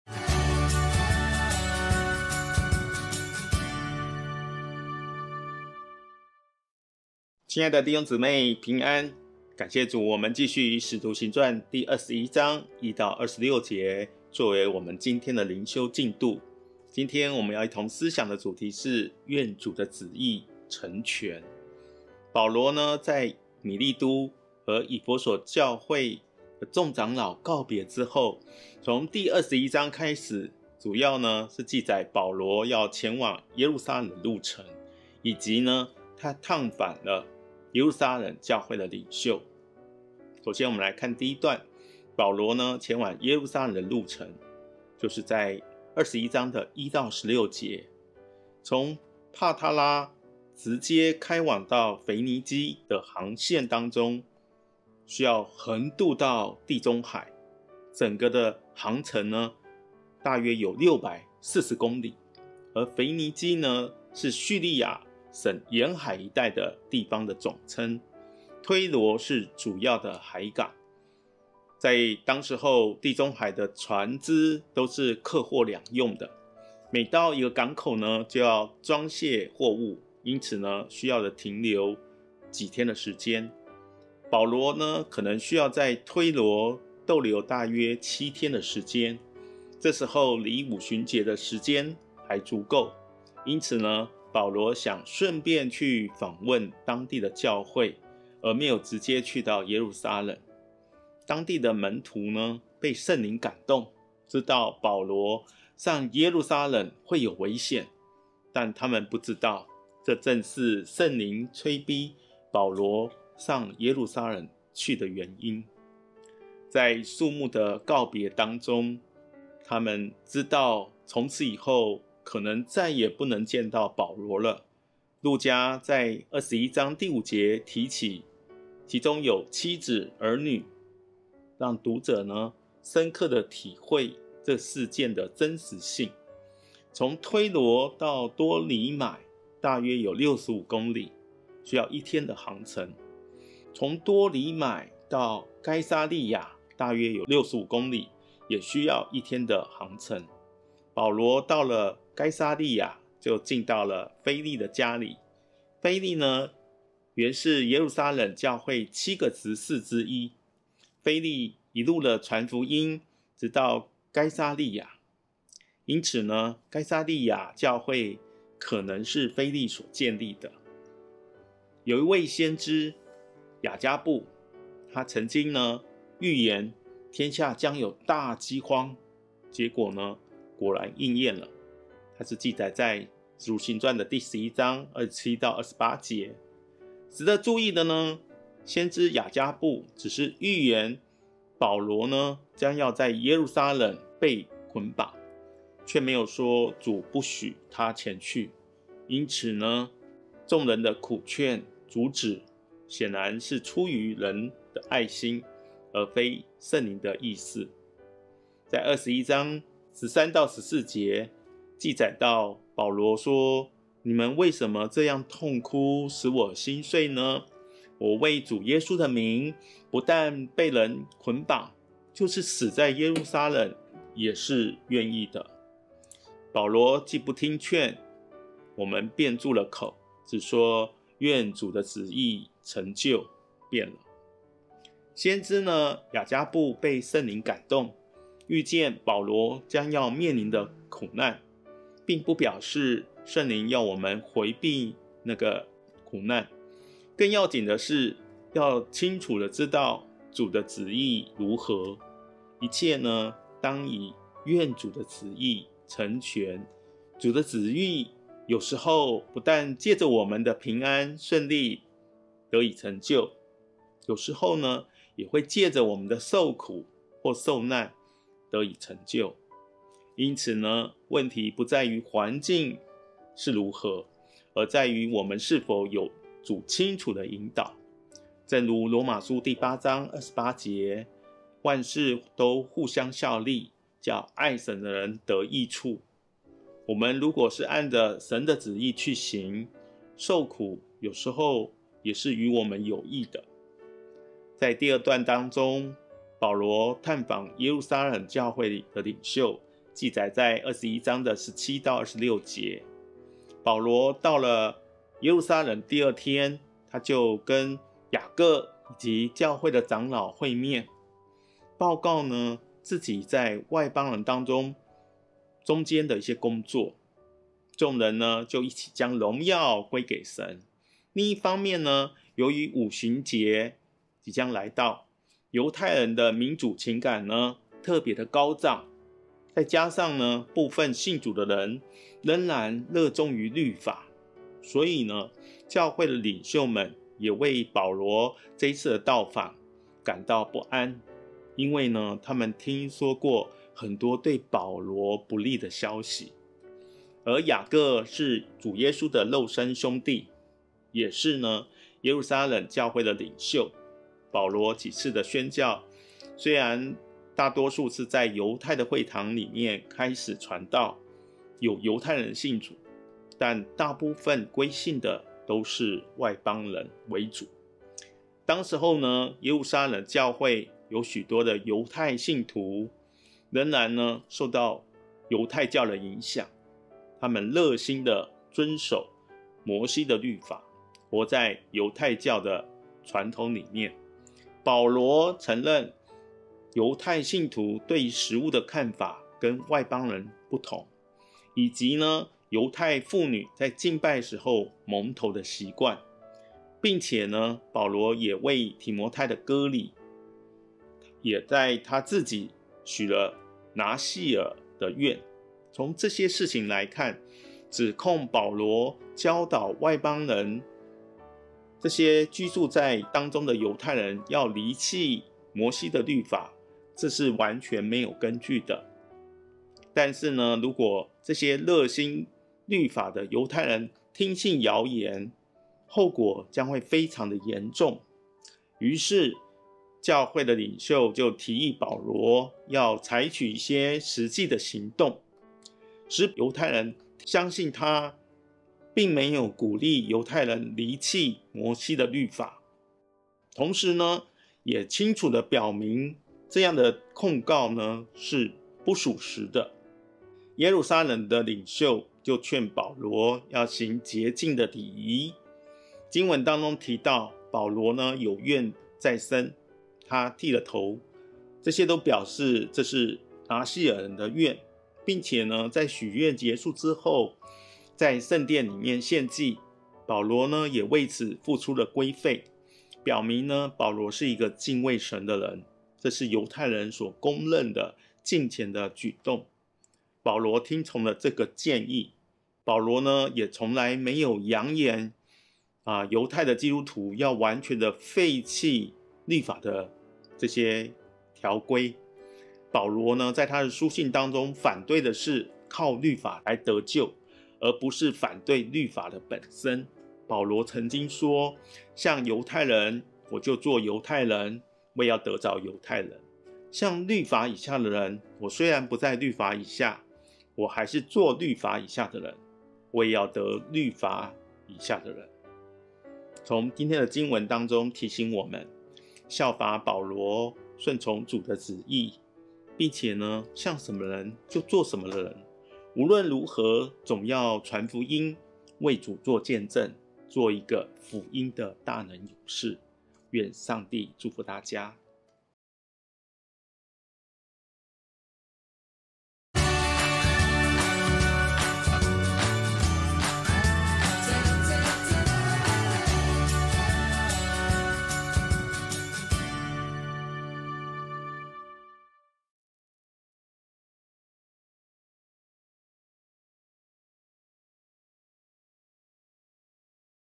《使徒行傳》21:1-26｜第三次宣教的結束｜每日靈修‧重新得力｜台北懷恩堂
親愛的弟兄姊妹平安！感謝主！我們繼續以《使徒行傳》 21 章 1 到 26 節作為靈修的進度，今天我們要思想的主題是「願主的旨意成全」。

保羅在米利都和以弗所教會長老的告別之後，從第 21 章開始，主要記載保羅前往耶路撒冷的路程，以及探訪耶路撒冷教會的領袖。

首先我們來看第一段，保羅前往耶路撒冷的路程，就是在 21 章的 1 到 16 節。從帕大喇直接開往腓尼基的航線，需要橫渡地中海，整個航程約有 640 公里，腓尼基是敘利亞省沿海一帶地方的總稱，推羅是主要的海港。古時候地中海的船隻都是客貨兩用，每到一個港口就要裝卸貨物，因此需要停留幾天。

保羅可能需要在推羅逗留七天，這時離五旬節的時間還足夠，因此保羅想順便去訪問當地的教會，而沒有直接去耶路撒冷。當地的門徒被聖靈感動，知道保羅上耶路撒冷會有危險，但他們不知道，正是聖靈催逼保羅上耶路撒冷去原因。肅穆的告別，表示他們知道從此以後可能再也不能見到保羅了。路加在 21 章 5 節提起「妻子兒女」，讓讀者深刻體會事件的真實性。

從推羅到多利買大約 65 公里，需要一天的航程，從多利買到該撒利亞大約 65 公里，也需要一天的航程。保羅到了該撒利亞就進了腓利家裡，腓利原是耶路撒冷教會七個執事之一，腓利一路傳福音，直到該撒利亞，因此，該撒利亞教會可能是腓利所建立的。

有一位先知亞迦布曾經預言天下將有大饑荒，結果果然應驗了，他是記載在《使徒行傳》的第 11 章 27 到 28 節，值得注意的，先知亞迦布只是預言保羅將要在耶路撒冷被捆綁，卻沒有說主不許他前去，因此眾人的苦勸阻止，顯然是出於人的愛心，而非聖靈的意思。

21 章 13 到 14 節記載：

    《使徒行傳》21:13-14
    13 保羅說：「你們為甚麼這樣痛哭，使我心碎呢？我為主耶穌的名，不但被人捆綁，就是死在耶路撒冷也是願意的。」 14 保羅既不聽勸，我們便住了口，只說：「願主的旨意成就」，便了。

先知亞迦布被聖靈感動，預見保羅將要面臨的苦難，並不表示聖靈要我們迴避那困境，更要緊的是，要清楚知道主的旨意如何，一切當以「願主的旨意成全」。主的旨意有時候不但是藉著我們的平安順利而得以成就，有時候也會藉著我們的受苦與受難得以成就，因此問題不在於環境如何，而在於我們是否有主清楚的引導，正如 《羅馬書》第 8 章 28 節：

    《羅馬書》8:28
    28 我們曉得萬事都互相效力，叫愛神的人得益處，就是按祂旨意被召的人。

我們若是按著神的旨意去行，受苦也是與我們有益的。

在第二段當中，保羅探訪耶路撒冷教會領，記載在 21 章的 17 到 26 節。保羅到達耶路撒冷的第二天，就和雅各、以及教會的長老會面，報告自己在外邦人中間的工作，眾人一起將榮耀歸給神。另一方面，由於五旬節臨近，猶太人的民族情感特別高漲，再加上部分信主的人仍然熱衷於律法，所以教會領袖們也為保羅的到訪感到不安，因為他們聽過很多對保羅不利的消息。

雅各是主耶穌的肉身兄弟，也是耶路撒冷教會的領袖。保羅幾次的宣教，雖然在猶太會堂開始傳道，有猶太人信主，但大部分歸信的都是外邦人。

當時耶路撒冷教會的許多猶太信徒，仍然深受猶太教的影響，熱心遵守摩西的律法，活在猶太教的傳統裡。

保羅承認猶太信徒對食物的看法和外邦人不同，以及猶太婦女敬拜時蒙頭的習慣，並且保羅也為提摩太行割禮，而他自己也許了拿細耳的願，從這些事情看來，指控保羅教導在外邦地方居住的猶太人，要離棄摩西的律法，這是完全沒有根據的，但是如果熱心律法的猶太人聽信謠言，後果會非常地嚴重，於是教會領袖提議保羅要採取一些實際行動，使猶太人相信他並沒有鼓勵猶太人離棄摩西的律法，同時也清楚表明這樣的控告是不屬實的。耶路撒冷的領袖勸保羅行潔淨禮儀，經文提到保羅有願在身和剃了頭，都是表示這是拿細耳人的願，並且在許願結束後，在聖殿獻祭。保羅為此付出規費，表明保羅是一個敬畏神的人，這是猶太人所公認的敬虔舉動，保羅聽從了此建議。保羅從未揚言猶太基督徒要完全廢棄律法的條規，保羅在他的書信中反對的是，靠律法得救，而非反對律法本身。保羅曾說：

    《哥林多前書》9:20
    20 向猶太人，我就作猶太人，為要得猶太人；向律法以下的人，我雖不在律法以下，還是作律法以下的人，為要得律法以下的人。

從今天的經文提醒我們，效法保羅順從主的旨意，並且向什麼人，就作什麼人，無論如何，總要傳福音，為主作見證，做一個福音的大能勇士。

願上帝祝福大家！

經文：使徒行傳 21:1-26

1我們離別了眾人，就開船一直行到哥士。第二天到了羅底，從那裏到帕大喇， 2遇見一隻船要往腓尼基去，就上船起行。 3望見塞浦路斯，就從南邊行過，往敘利亞去，我們就在泰爾上岸，因為船要在那裏卸貨。 4找着了門徒，就在那裏住了七天。他們被聖靈感動，對保羅說：「不要上耶路撒冷去。」 5過了這幾天，我們就起身前行。他們眾人同妻子兒女，送我們到城外，我們都跪在岸上禱告，彼此辭別。

6我們上了船，他們就回家去了。

7我們從泰爾行盡了水路，來到多利買，就問那裏的弟兄安，和他們同住了一天。 8第二天，我們離開那裏，來到凱撒利亞，就進了傳福音的腓利家裏，和他同住。他是那七個執事裏的一個。 9他有四個女兒，都是處女，是說預言的。 10我們在那裏多住了幾天，有一個先知，名叫亞迦布，從猶太下來， 11到了我們這裏，就拿保羅的腰帶捆上自己的手腳，說：「聖靈說：猶太人在耶路撒冷，要如此捆綁這腰帶的主人，把他交在外邦人手裏。」 12我們和那本地的人聽見這話，都苦勸保羅不要上耶路撒冷去。 13保羅說：「你們為甚麼這樣痛哭，使我心碎呢？我為主耶穌的名，不但被人捆綁，就是死在耶路撒冷也是願意的。」 14保羅既不聽勸，我們便住了口，只說：「願主的旨意成就」，便了。

15過了幾日，我們收拾行李上耶路撒冷去。 16有凱撒利亞的幾個門徒和我們同去，帶我們到一個久為門徒的家裏，叫我們與他同住；他名叫拿孫，是塞浦路斯人。

17到了耶路撒冷，弟兄們歡歡喜喜地接待我們。 18第二天，保羅同我們去見雅各；長老們也都在那裏。 19保羅問了他們安，便將神用他傳教，在外邦人中間所行之事，一一地述說了。 20他們聽見，就歸榮耀與神，對保羅說：「兄台，你看猶太人中信主的有多少萬，並且都為律法熱心。 21他們聽見人說，你教訓一切在外邦的猶太人離棄摩西，對他們說，不要給孩子行割禮，也不要遵行條規。 22眾人必聽見你來了，這可怎麼辦呢？ 23你就照着我們的話行吧！ 我們這裏有四個人，都有願在身。 24你帶他們去，與他們一同行潔淨的禮，替他們拿出規費，叫他們得以剃頭。這樣，眾人就可知道，先前所聽見你的事都是虛的；並可知道，你自己為人，循規蹈矩，遵行律法。 25至於信主的外邦人，我們已經寫信擬定，叫他們謹忌那祭偶像之物，和血，並勒死的牲畜，與姦淫。」 26於是保羅帶着那四個人，第二天與他們一同行了潔淨的禮，進了殿，報明潔淨的日期滿足，只等祭司為他們各人獻祭。